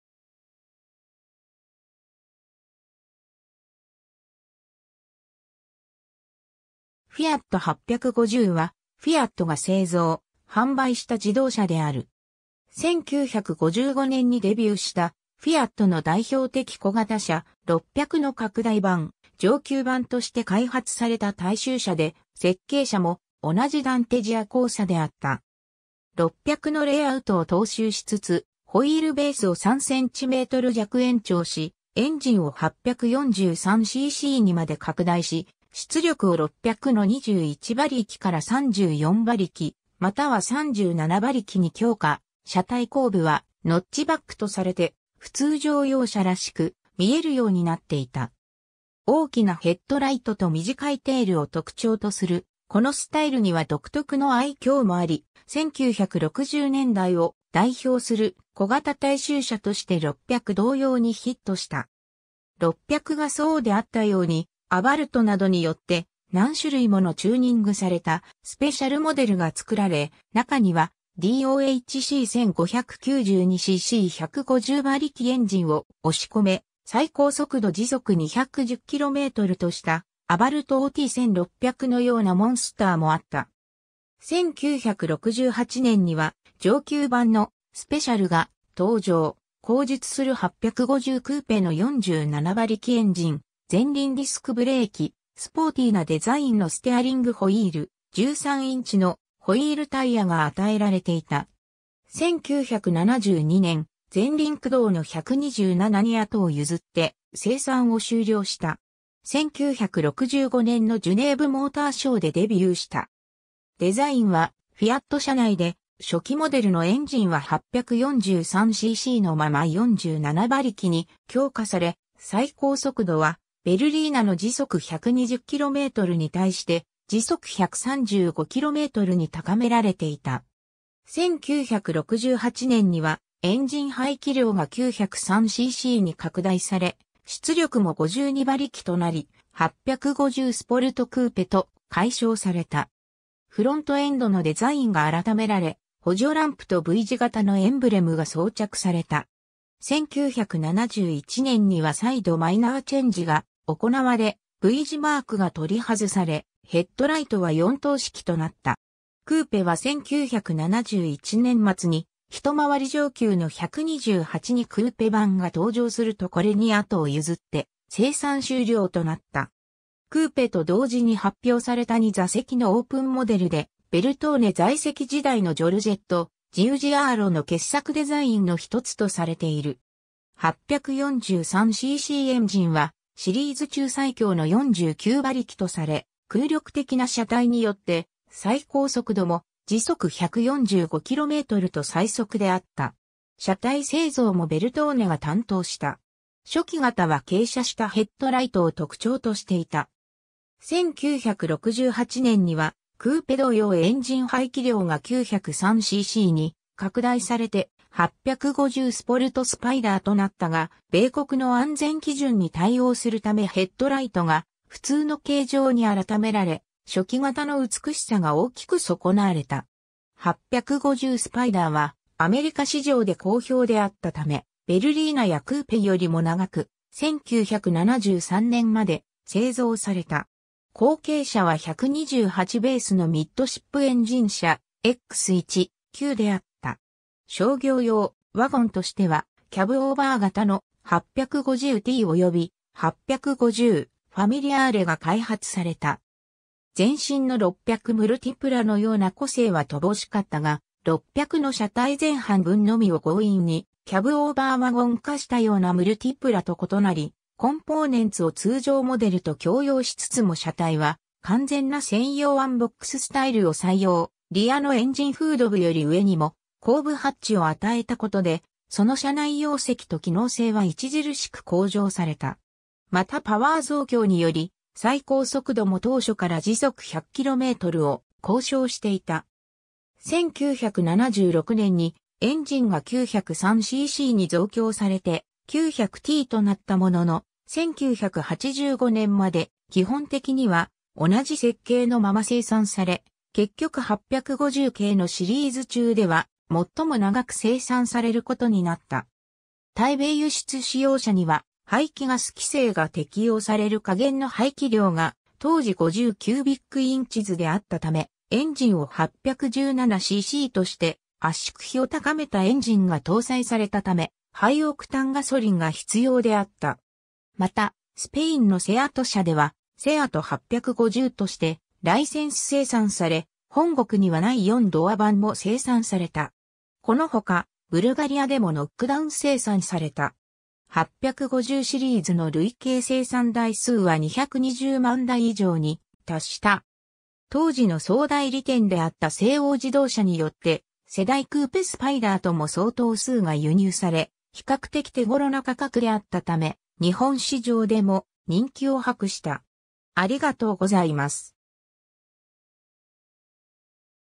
フィアット・850
フィアット・850はフィアットが製造・販売した自動車である。
 1955年にデビューしたフィアットの代表的小型車・600の拡大版・上級版として開発された大衆車で、設計者も同じダンテ・ジアコーサであった。
600のレイアウトを踏襲しつつ、ホイールベースを3cm弱延長し、エンジンを843ccにまで拡大し、出力を600の21馬力から34馬力(ノルマーレ)または37馬力(スーパー)に強化、車体後部はノッチバックとされて普通乗用車らしく見えるようになっていた。
大きなヘッドライトと短いテールを特徴とするこのスタイルには独特の愛嬌もあり、1960年代を代表する小型大衆車として600同様にヒットした。
600がそうであったように、アバルトなどによって、何種類ものチューニングされたスペシャルモデルが作られ、中にはDOHC1592cc150馬力エンジンを押し込め、最高速度210km/hとした「アバルトOT1600」のようなモンスターもあった。
 1968年には上級版の「スペシャル」が登場、後述する850クーペの47馬力エンジン、前輪ディスクブレーキ、スポーティーなデザインのステアリングホイール、13インチのホイール・タイヤが与えられていた。
 1972年、前輪駆動の127に後を譲って生産を終了した。
 1965年のジュネーヴモーターショーでデビューした。
デザインはフィアット社内で、初期モデルのエンジンは843ccのまま47馬力に強化され、最高速度はベルリーナの120km/hに対して135km/hに高められていた。
1968年にはエンジン排気量が903ccに拡大され、出力も52馬力となり「850スポルト・クーペ」と改称された。
フロントエンドのデザインが改められ、補助ランプとV字型のエンブレムが装着された。
1971年には再度マイナーチェンジが行われ、V字マークが取り外され、ヘッドライトは4灯式となった。
クーペは1971年末に、一回り上級の128にクーペ版が登場するとこれに後を譲って生産終了となった。
 クーペと同時に発表された2座席のオープンモデルで、ベルトーネ在籍時代のジョルジェット・ジウジアーロの傑作デザインのひとつとされている。
843ccエンジンはシリーズ中最強の49馬力とされ、空力的な車体によって最高速度も145km/hと最速であった。
車体製造もベルトーネが担当した。
初期型は傾斜したヘッドライトを特徴としていた。
1968年にはクーペ同様エンジン排気量が903ccに拡大されて「850スポルト・スパイダー」となったが、米国の安全基準に対応するためヘッドライトが普通の形状に改められ、初期型の美しさが大きく損なわれた。
 850スパイダーはアメリカ市場で好評であったため、ベルリーナやクーペよりも長く、1973年まで製造された。
後継者は128ベースのミッドシップエンジン車・X1/9であった。
 商業用・ワゴンとしてはキャブオーバー型の「850T」（貨物バンモデル）および「850ファミリアーレ」（乗用モデル　標準で7人乗り）が開発された。
前身の600ムルティプラのような個性は乏しかったが、600の車体前半分のみを強引にキャブオーバーワゴン化したようなムルティプラと異なり、コンポーネンツを通常モデルと共用しつつも車体は完全な専用ワンボックススタイルを採用、リアのエンジンフード部より上にも後部ハッチを与えたことで、その車内容積と機能性は著しく向上された。
またパワー増強により、最高速度も当初から100km/hを公称していた。
1976年にエンジンが903ccに増強されて「900T」となったものの、1985年まで基本的には同じ設計のまま生産され、結局850系のシリーズ中ではもっとも長く生産されることになった。
 対米輸出仕様車には、排気ガス規制が適用される下限の排気量が当時50 cubic inchesであったため、エンジンを817cc(49.8ci)として圧縮比を高めたエンジンが搭載されたため、ハイオクタンガソリンが必要であった。
また、スペインのセアト社では「セアト・850」としてライセンス生産され、本国にはない4ドア版も生産された。
この他、ブルガリアでもノックダウン生産された。
 850シリーズの累計生産台数は220万台以上に達した。
 当時の総代理店であった西欧自動車によってセダン・クーペ・スパイダーとも相当数が輸入され、比較的手ごろな価格（100万円台前半で、当時のトヨタ・クラウンの高級モデルとほぼ同じ価格帯）であったため、日本市場でも人気を...